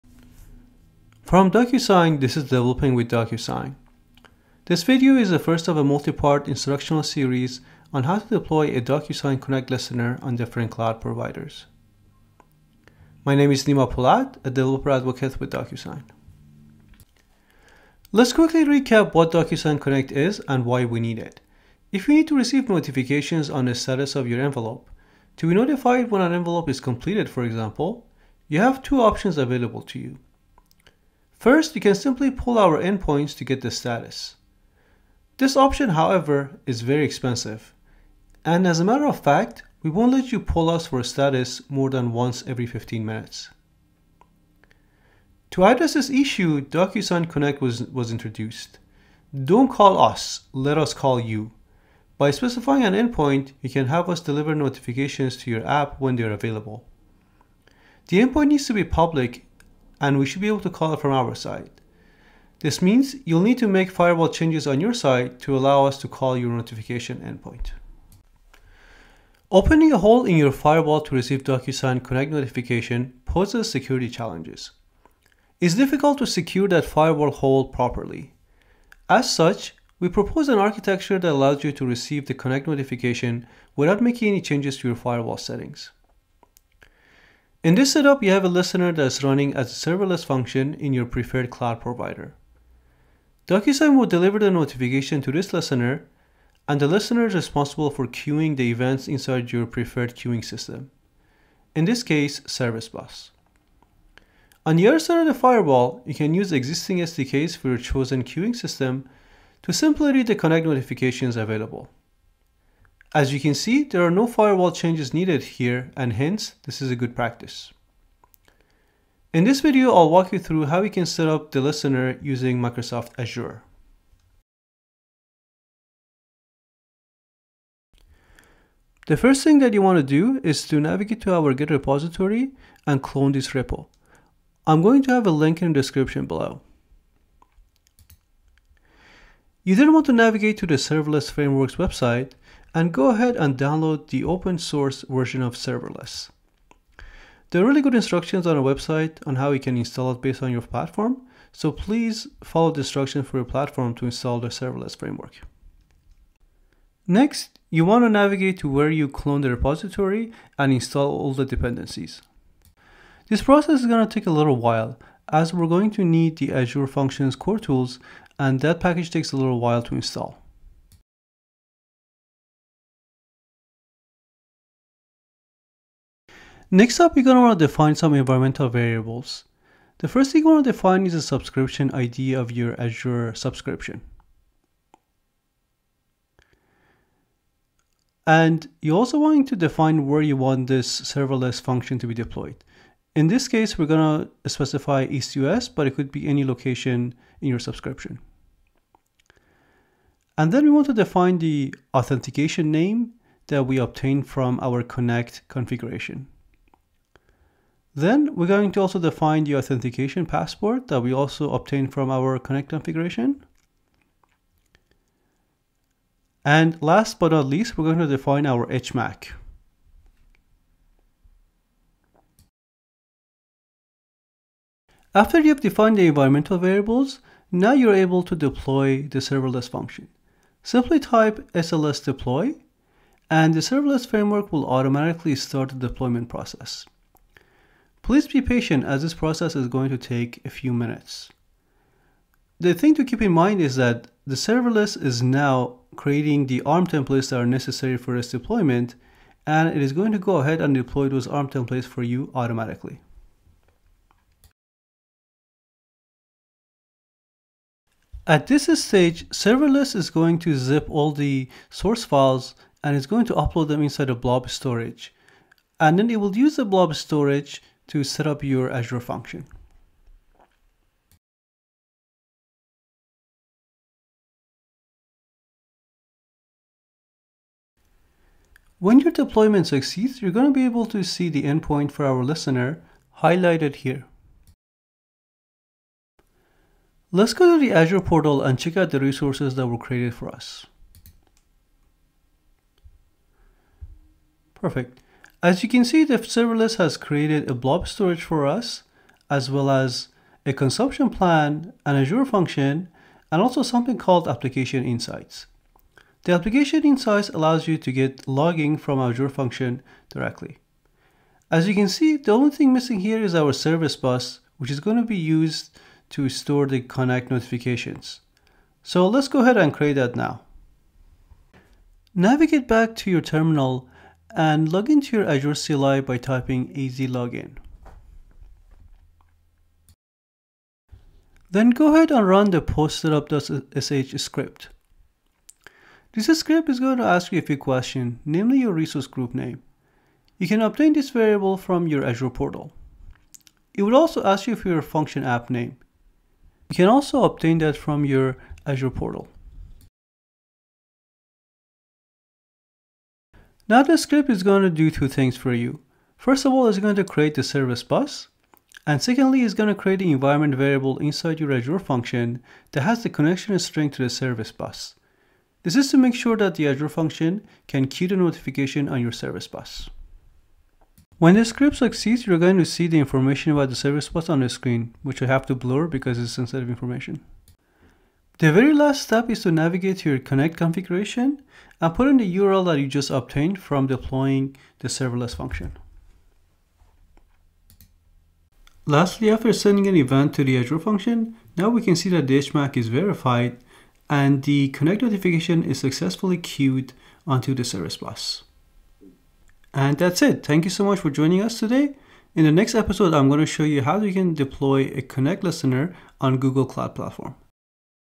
Deploy DocuSign Connect Listener on Azure | Developer Education
DocuSign Connect allows you to receive notifications about the status of your envelope. In this multi-part instructional series, we will explore how to deploy a DocuSign Connect Listener using Microsoft Azure.